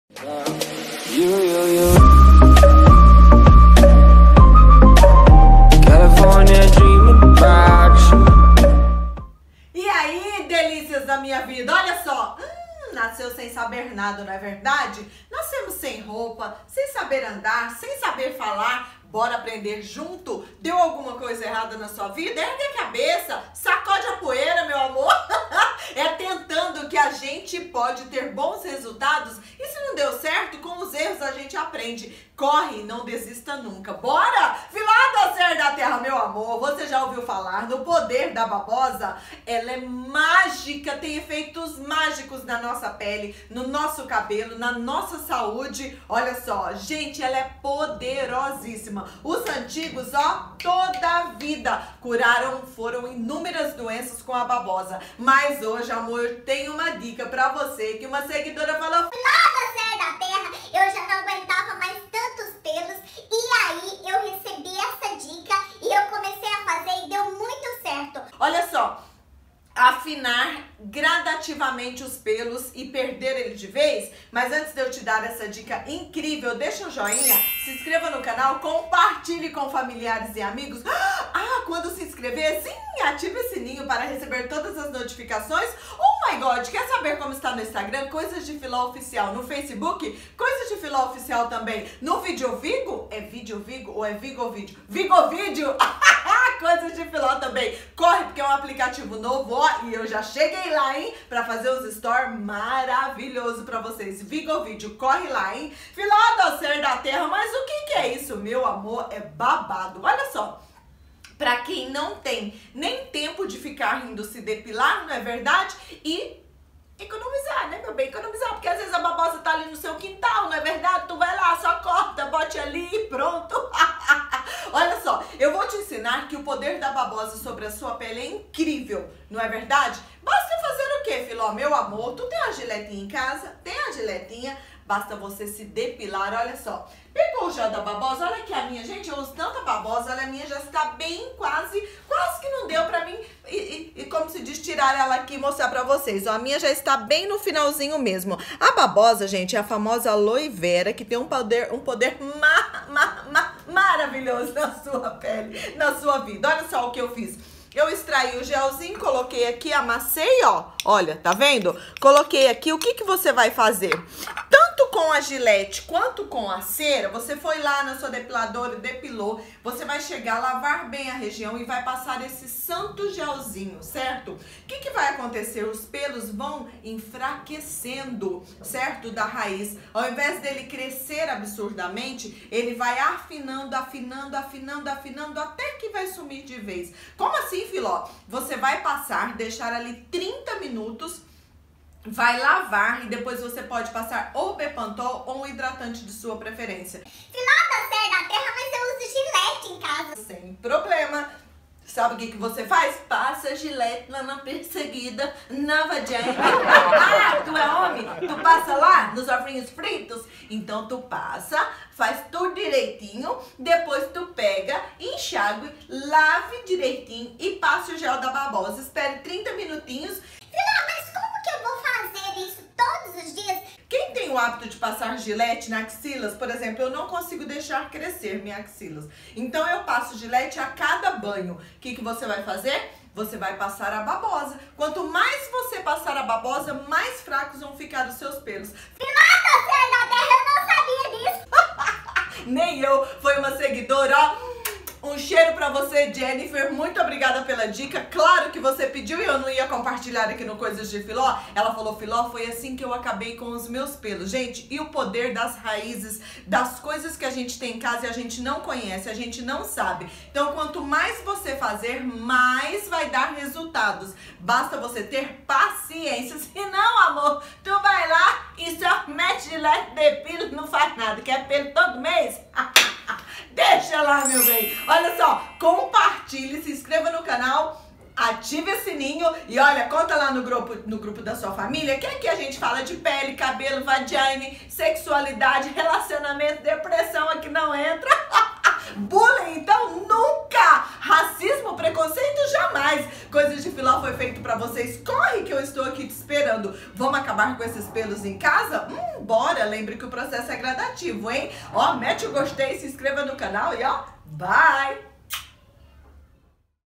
PARE DE DEPILAR JÁ! Use BABOSA + Isso para Remover PELOS Sem DOR depilação
PARE DE DEPILAR JÁ! Use BABOSA + Isso para Remover PELOS Sem DOR coisasdefiló depilação
OS 100 MELHORES ALIMENTOS ANTIENVELHECIMENTO